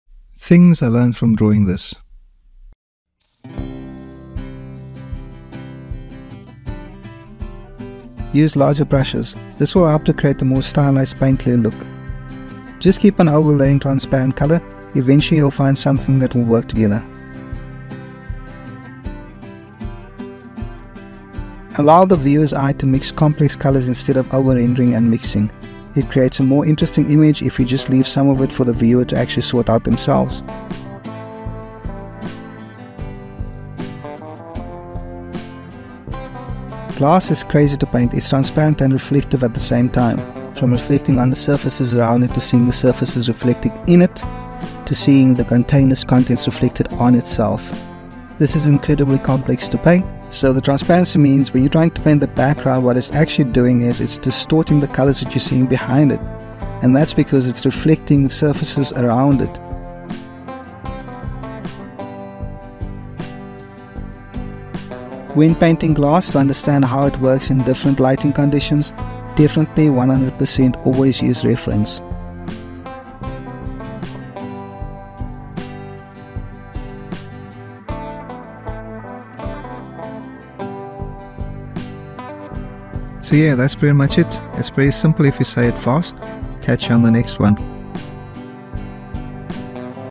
Digital Drawing Tips - Whiskey
Drawing glass and reflections are difficult, but keen observation and good references can help you create visually correct aesthetics.